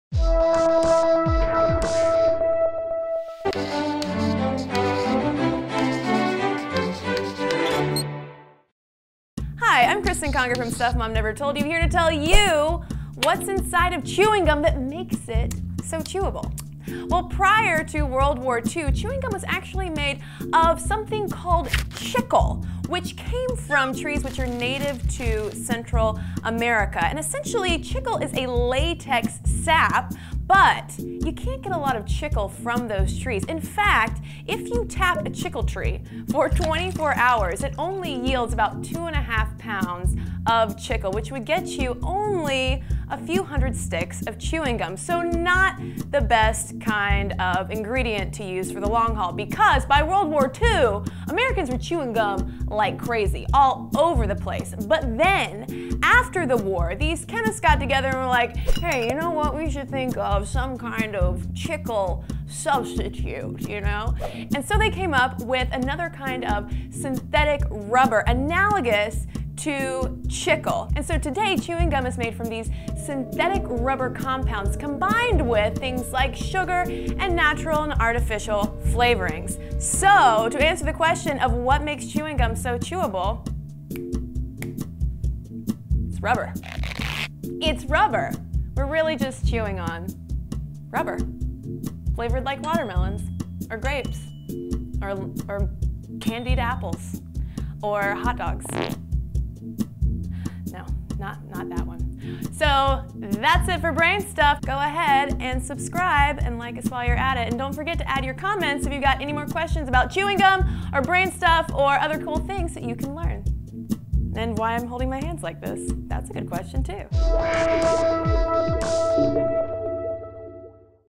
What Is Chewing Gum Made Of?
Up until World War II, chewing gum was made of a substance called chicle. Check out this Brain Stuff episode to learn more about the ingredients of modern chewing gum.